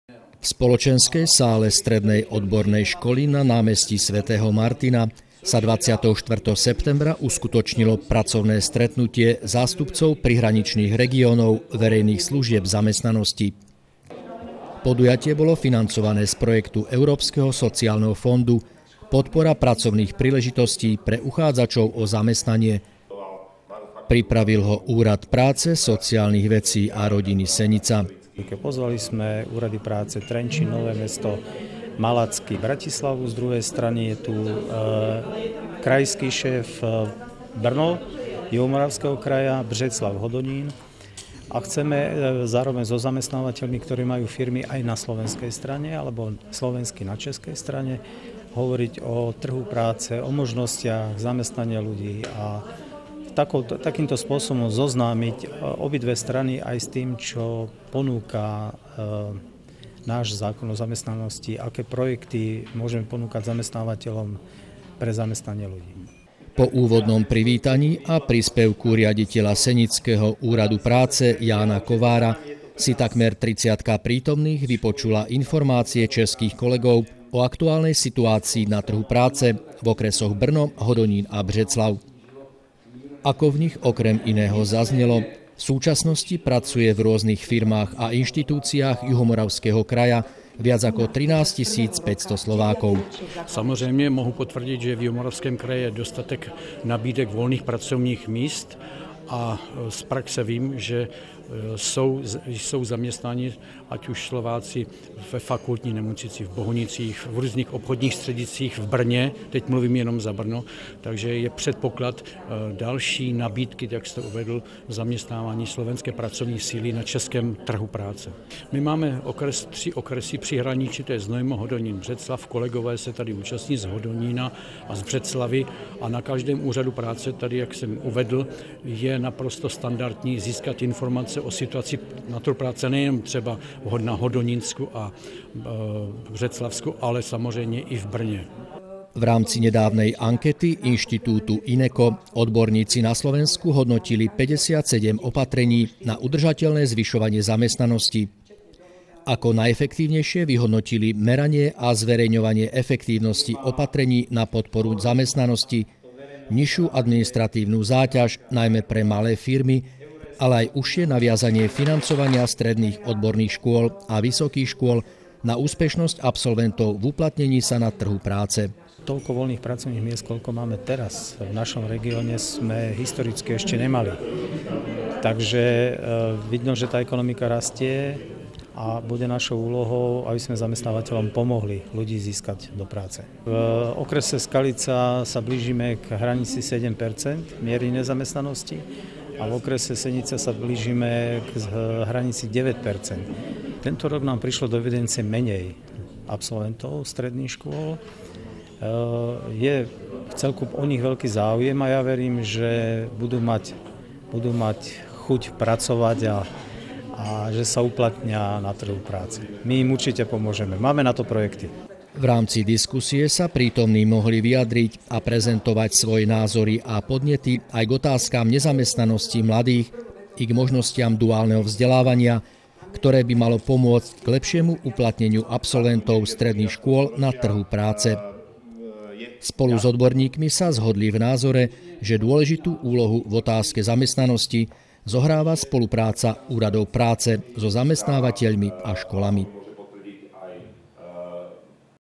Zástupcovia prihraničných regiónov verejných služieb rokovali o otázkach zamestnanosti
1698